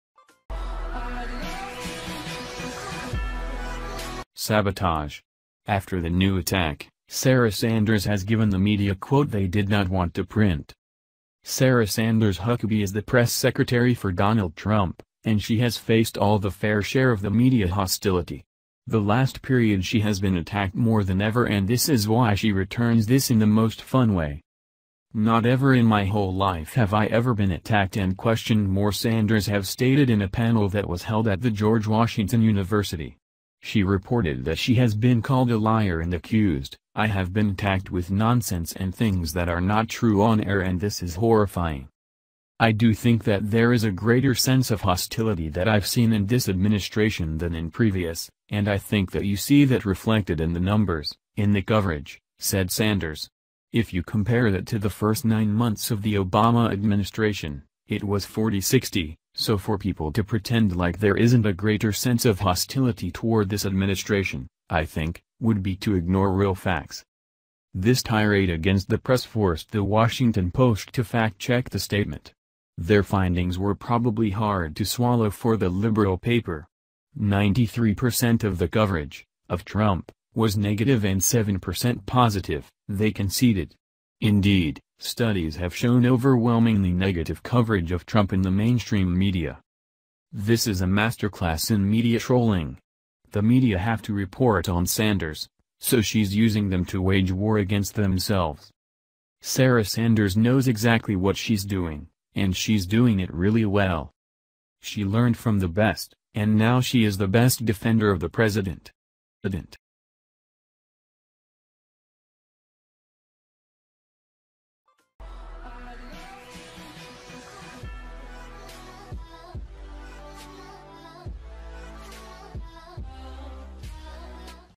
SABOTAGE! After The New Attack, Sarah Sanders Has Given The Media Quote They Did NOT Want To Print
SABOTAGE! After The New Attack, Sarah Sanders Has Given The Media Quote They Did NOT Want To Print - POLITICS NEWS
Sarah Sanders Huckabee is the Press Secretary for Donald Trump, and she has faced all the fair share of the media hostility. The last period she has been attacked more than ever and this is why she returns this in the most fun way.

“Not ever in my whole life have I ever been attacked and questioned more” Sanders have stated in a panel that was held at the George Washington University. She reported that she has been called a liar and accused, “I have been attacked with nonsense and things that are not true on air and this is horrifying”.
“I do think that there is a greater sense of hostility that I’ve seen in this administration than in previous, and I think that you see that reflected in the numbers, in the coverage,” said Sanders. “If you compare that to the first nine months of the Obama administration, it was 40-60, so for people to pretend like there isn’t a greater sense of hostility toward this administration, I think, would be to ignore real facts.”

This tirade against the press forced the Washington Post to fact-check the statement. Their findings were probably hard to swallow for the liberal paper. “93 percent of the coverage [of Trump] was negative and 7 percent positive,” They conceded. “Indeed, studies have shown overwhelmingly negative coverage of Trump in the mainstream media.”

This is a MASTERCLASS in media trolling. The media have to report on Sanders, so she’s using them to wage war against themselves.

Sarah Sanders knows exactly what she’s doing — and she’s doing it REALLY well.

She learned from the best, and now she is the best defender of the president.